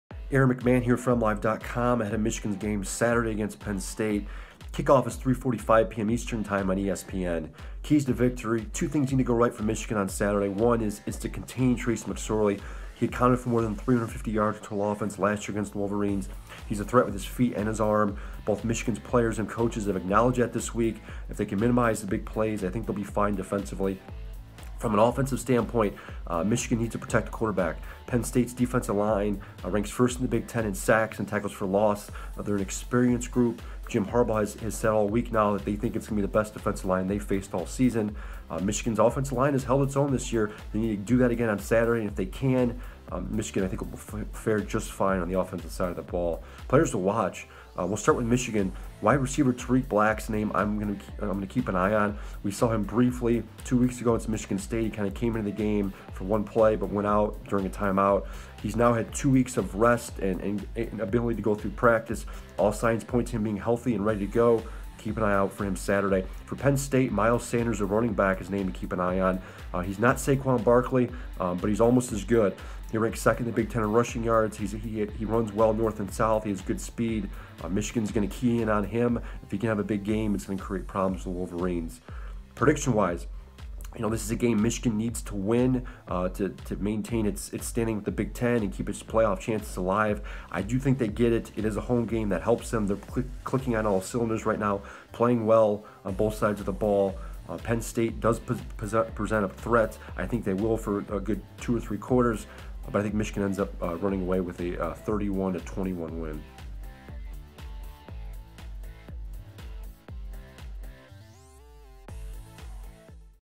Keys to Victory: Michigan Week 10
Michigan faces Penn State Saturday.